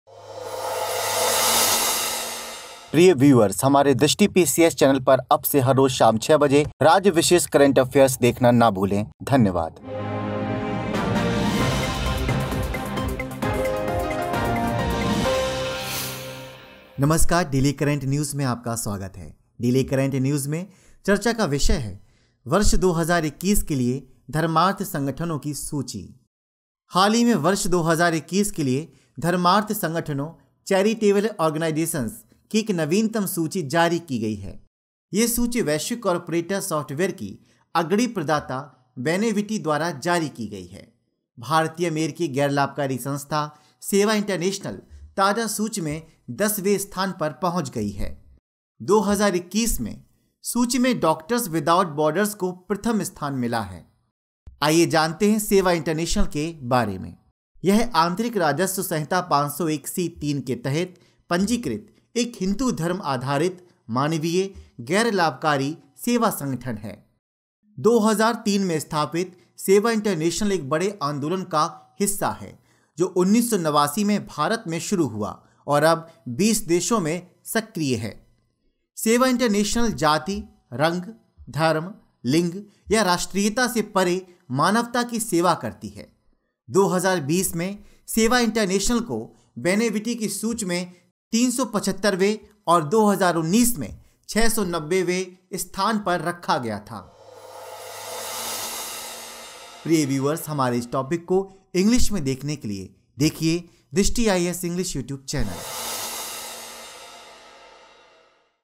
Indian-American NGO Jumps To 10th Spot Among Charitable Organizations Daily Current News Drishti IAS
Indian-American non-profit body Sewa International has jumped to the 10th spot in a latest list of charitable organizations for the year 2021, mainly because of the commendable work carried by it during last year's COVID-19 crisis in both India and the United States.

1. Videos by Dr. Vikas Divyakriti To

2. Concept Talk- By Dr. Vikas Divyakirti

6. To The Point- Special

8. UPSC PT Prelims Practice Quiz